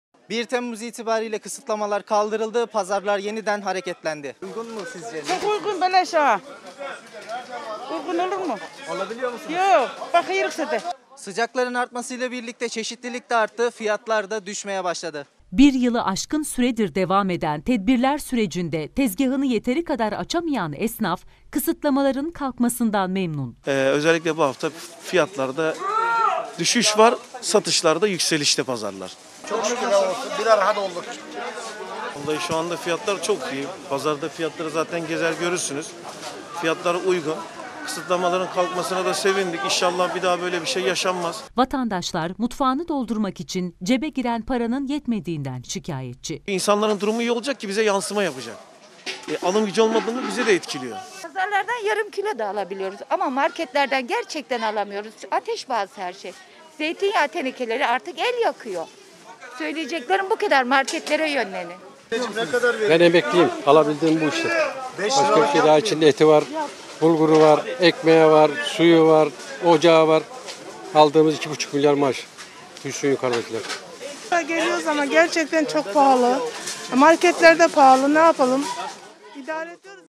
1 Temmuz'da çarşı pazarın durumu
Koronavirüs kısıtlamalarının sona ermesiyle birlikte pazarlarda hareketlilik yaşandı. Esnaf kısıtlamaların kalkmasından memnun olsa vatandaş yine alamamaktan şikayetçi.